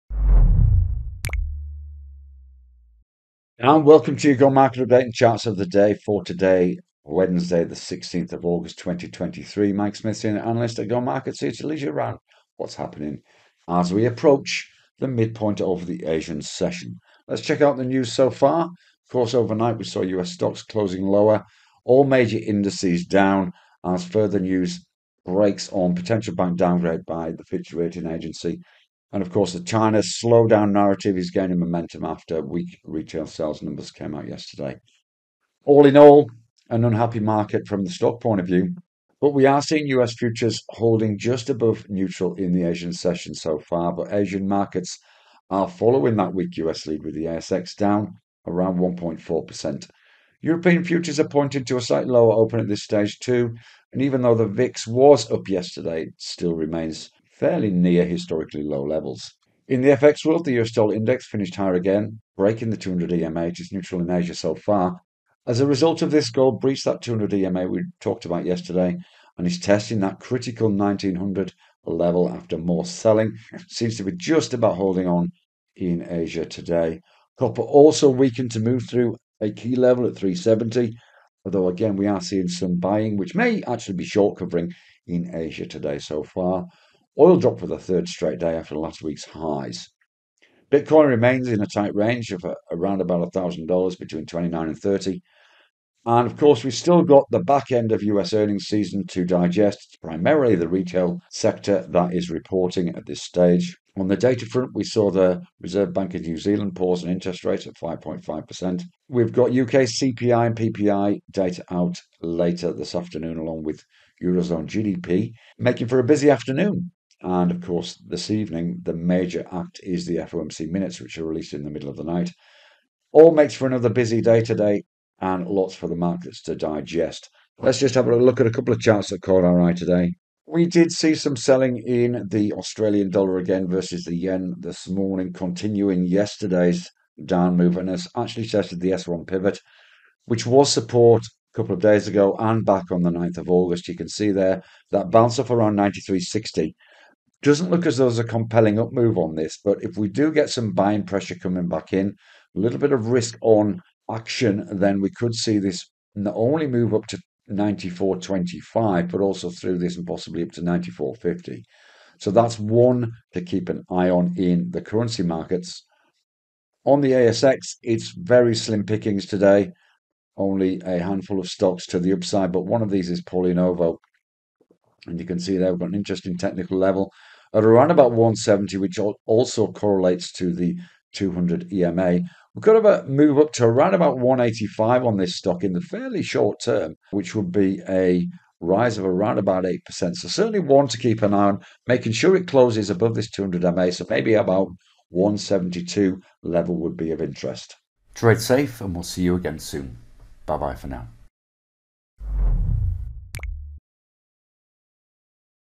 GO Market Update and Charts of Interest - PNV and AUDJPY
The recent rating agency created market twitch continued overnight in the US with a number of banks being put on watch for potential downgrade by Fitch. This combined with continued weak data out of China yesterday have the bulls under pressure both in stocks and commodities. Find out more..